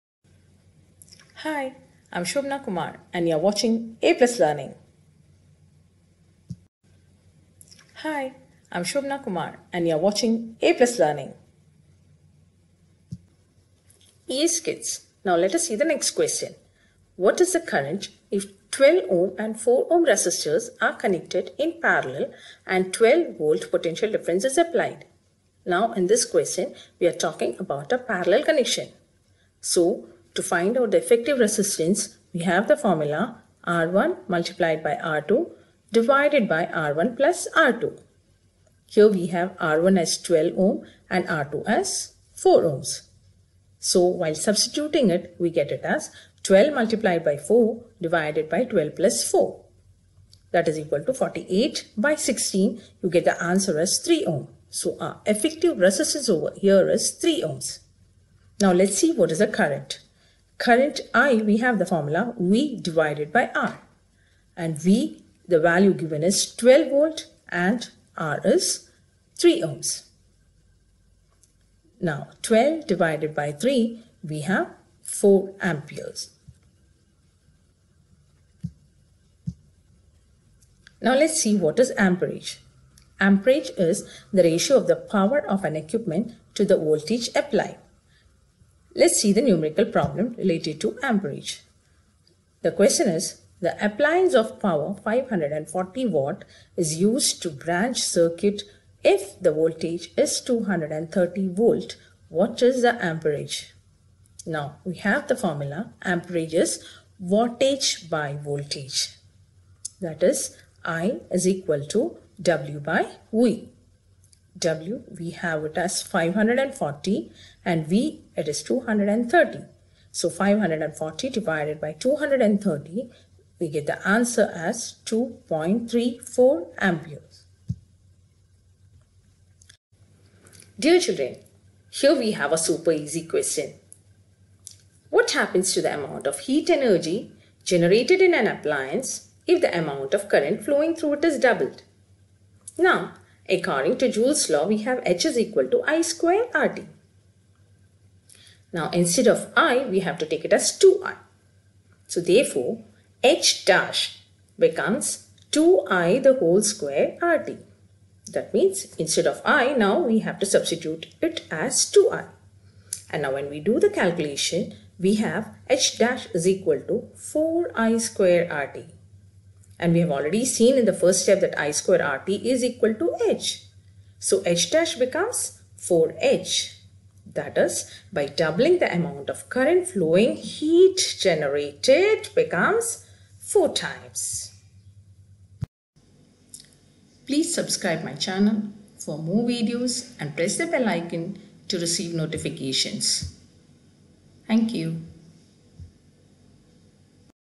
Effects of Electric Current solved numerical problems part 3 -Class 10
Grade 10 solved numerical problems of the chapter Effects of electric current.
Link for the Part 1 and Part 2 videos are given below